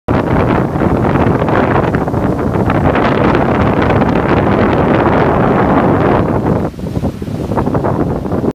blizzard in woods
Blizzard in the winter woods 2016, Long Island